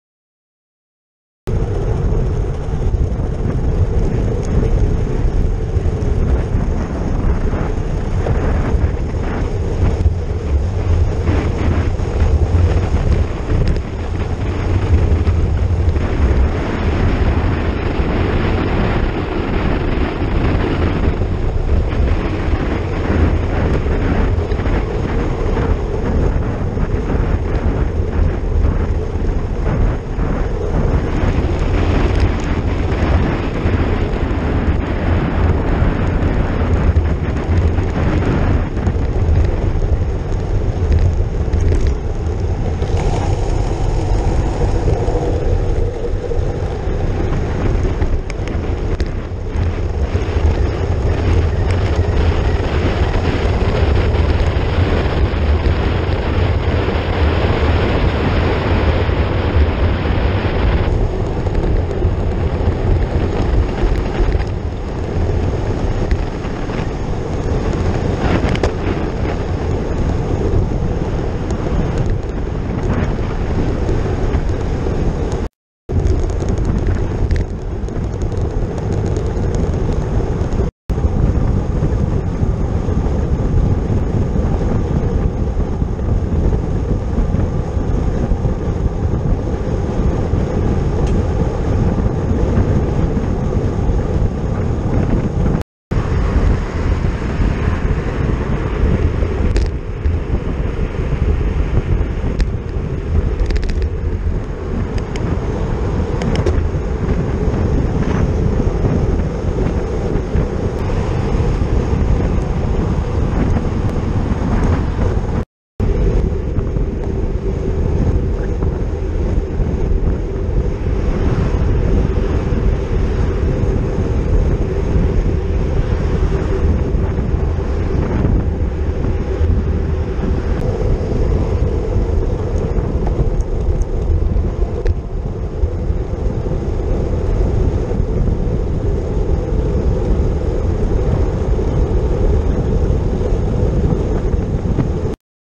Puech Auriol - Castres - - GoPro 1080p
1ère vidéo de test d'une GoPro Hero Session entre le Puech Auriol et Castres dans le sud du Tarn.
Cette vidéo est un premier test, soyez indulgent ! ;)